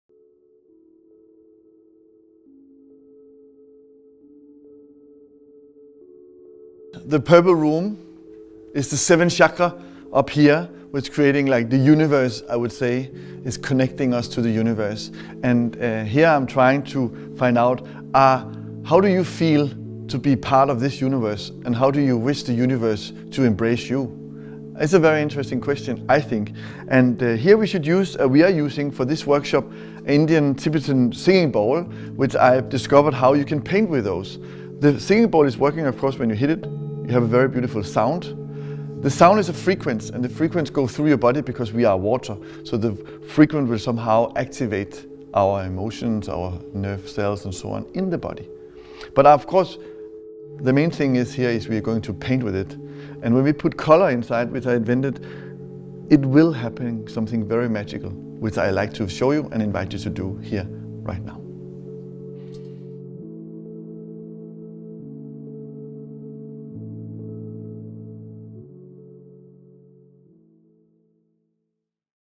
Jeppe Hein: Vem är du egentligen? | JAG FÖRSTÅR
Med frågan ”Vem är du egentligen?” vill konstnären Jeppe Hein att du som besöker utställningen ska få kontakt med de situationer han iscensätter.

Det lila ”Jag förstår”-rummet representerar det sjunde chakrat. Vi människor består till 70 procent av vatten som börjar vibrera vid vissa ljudfrekvenser. Prova på att måla med klangskålar och se hur vibrationerna syns i skålen.